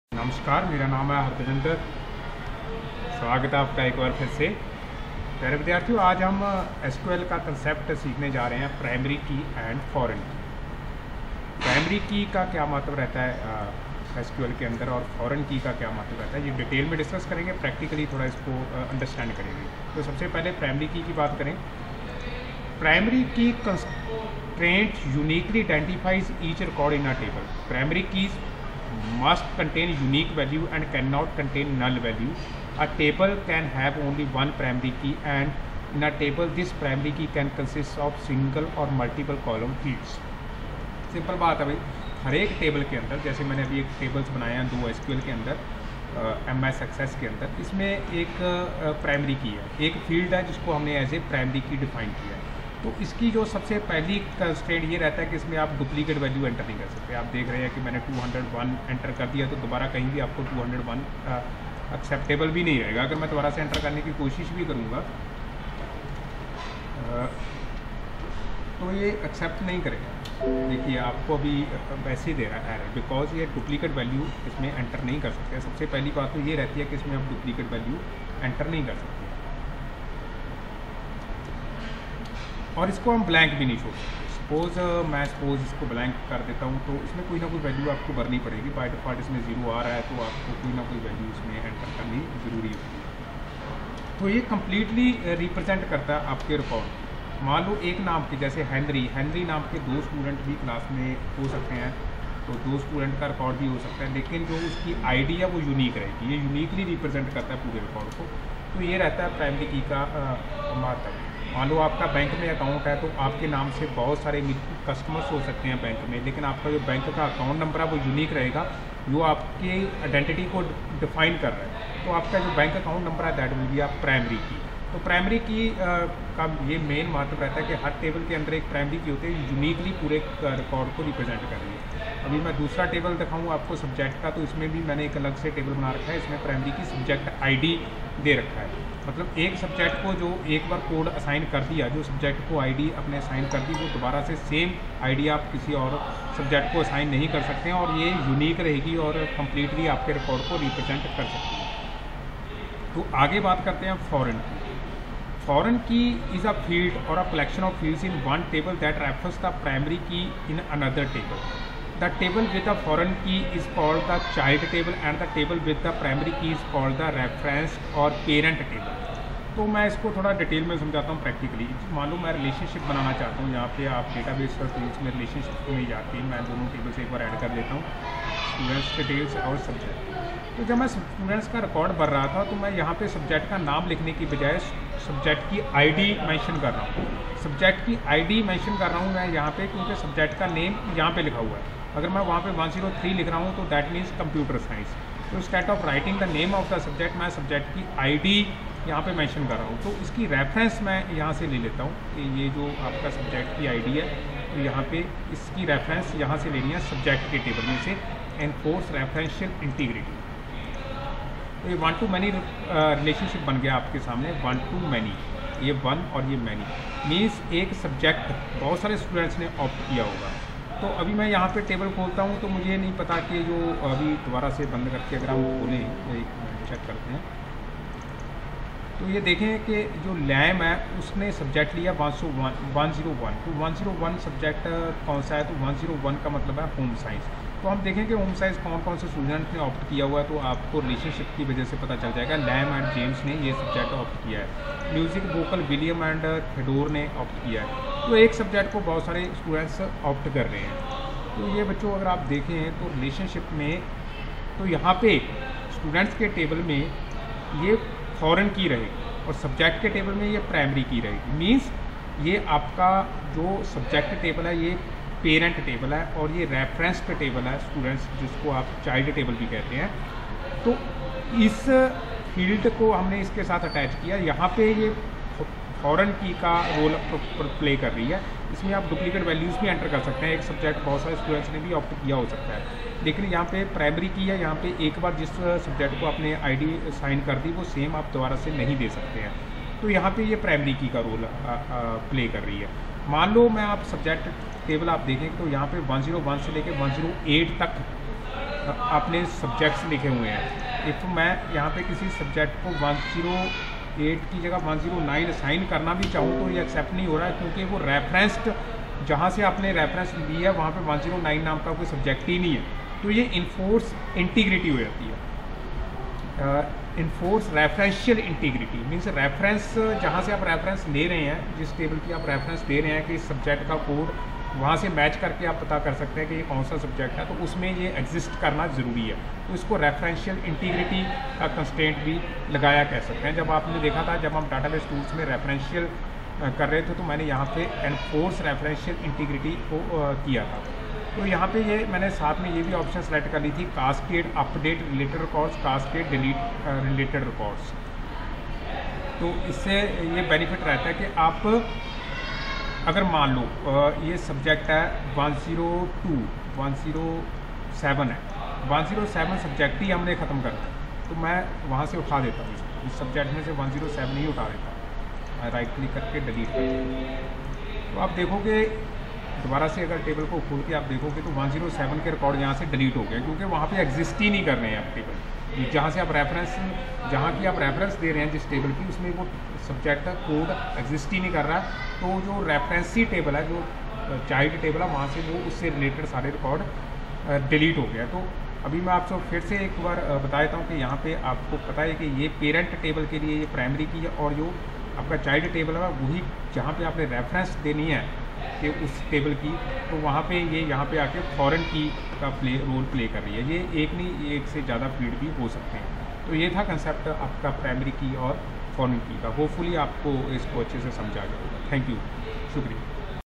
Primary Key and Foreign Key in SQL
This video illustrates the concept of primary key and foreign key in SQL. This video also explains Referential Integrity.